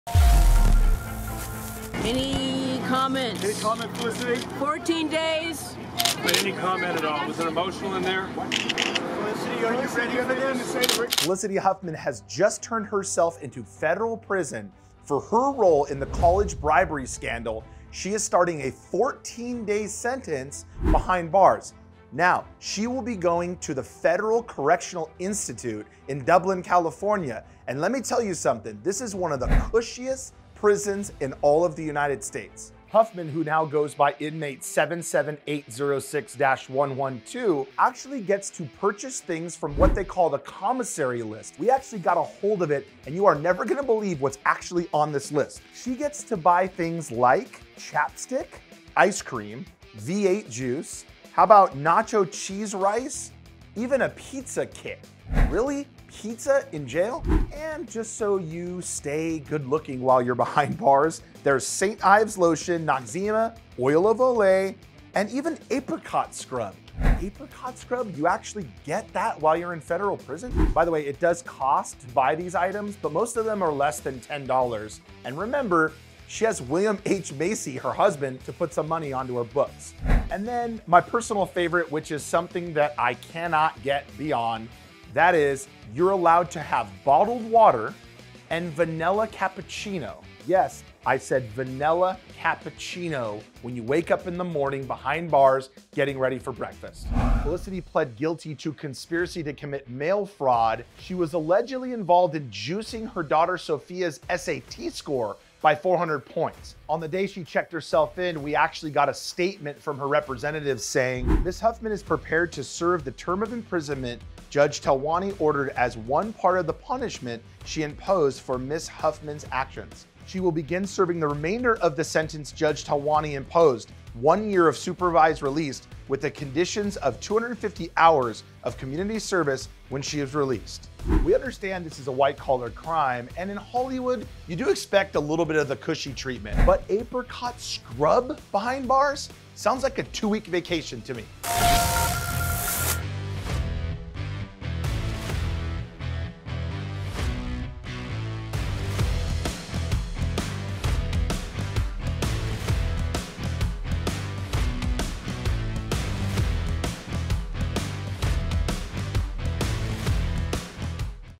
Felicity Huffman's Swanky Federal Prison Offers Cappuccino & Apricot Scrub
Felicity Huffman has just turned herself into a federal prison after the television star was sentenced to 14 days behind bars for her roll in the college bribery scandal.

Huffman will now go by Inmate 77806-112 in the Federal Correctional Institution in Dublin, California, which is about 40 miles east of San Francisco.